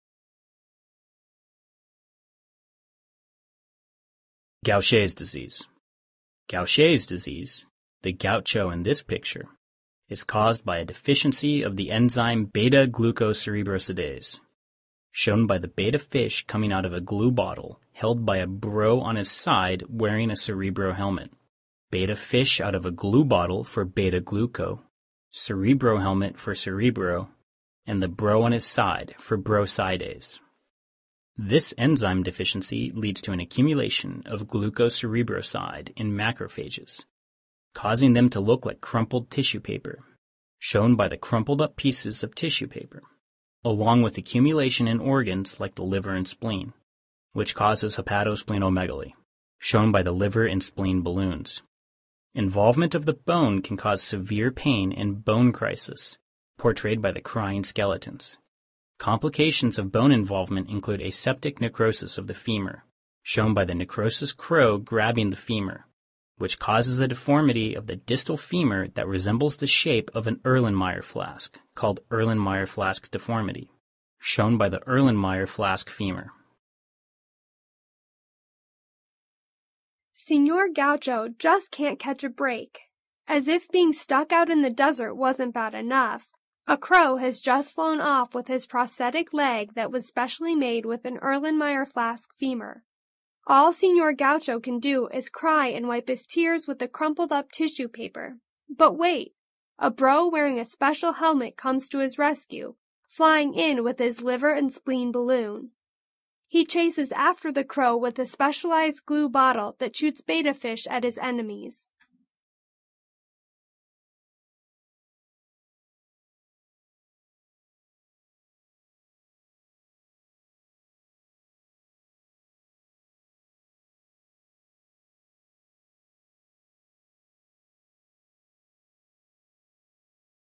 Gauchers Disease
Gaucher disease is a rare genetic disorder characterized by the deposition of glucocerebroside in cells of the macrophage-monocyte system. The disorder results from the deficiency of the enzyme glucocerebrosidase.
Copyrights to picmonic.